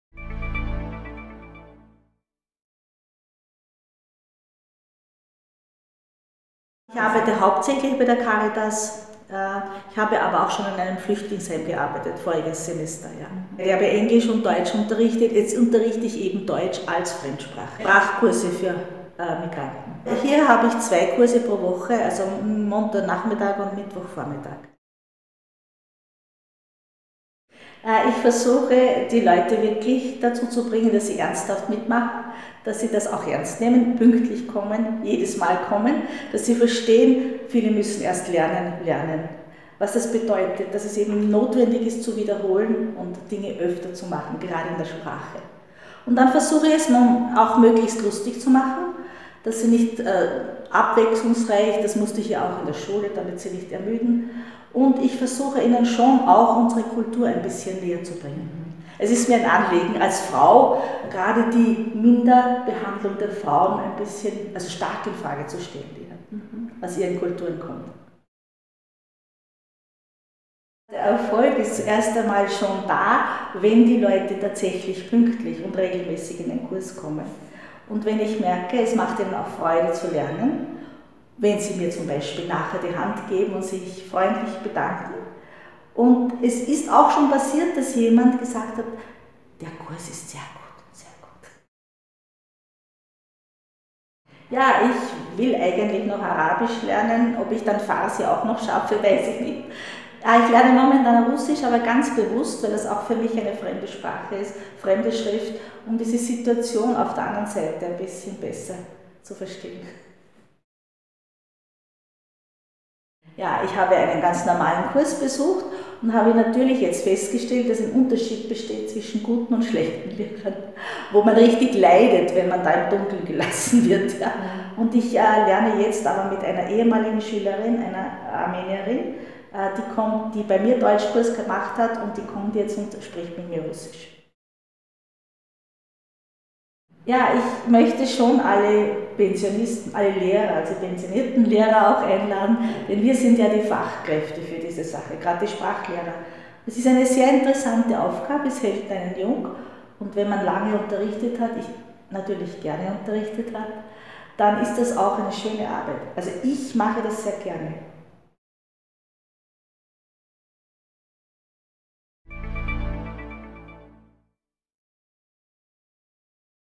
Das besondere Ehrenamt Erwachsenen lernen Lesen und Schreiben
Gerti Duma, eine ehrenamtliche Sprachbegleiterin der Caritas Wien, gibt in einem Video-Interview Auskunft darüber, was für sie Qualität in einem Kurs bedeutet, worauf sie besonders achtet, was sie als Erfolg bewertet, welche Sprachen sie selbst erlernt und wie sie das macht.